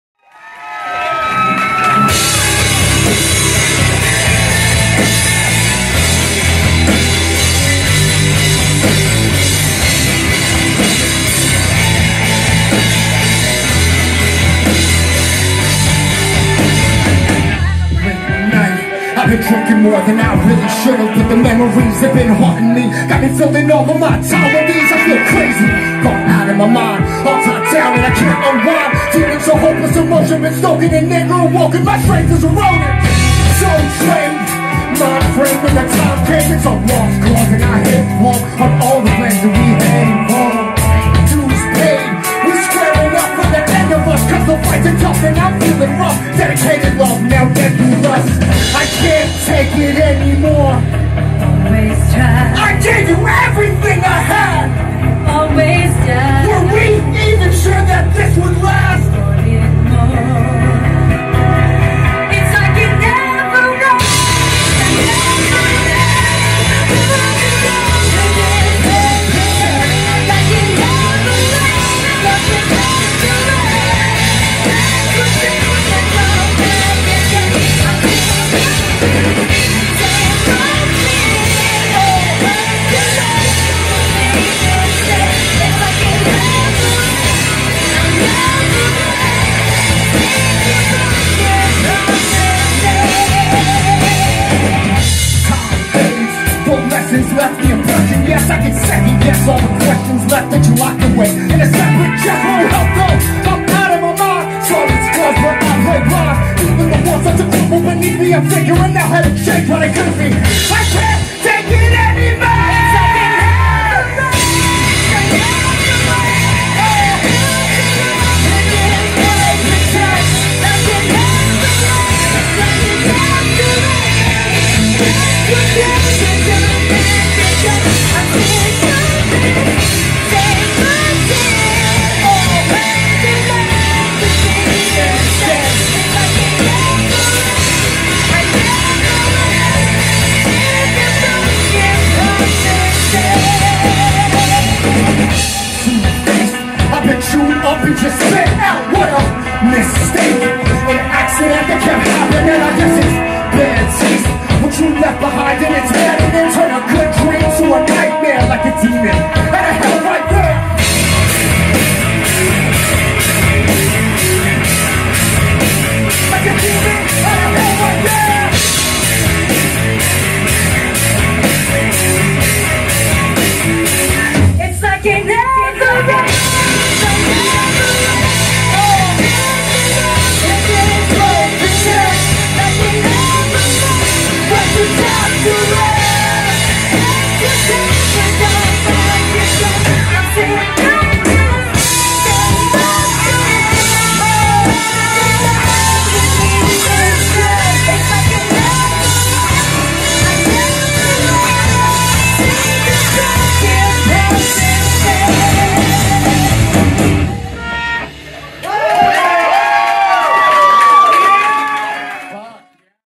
LYLVC @ Bad Habits 05 28 22
LYLVC performing at Bad Habits Bar and Grill in Martinsburg, West Virginia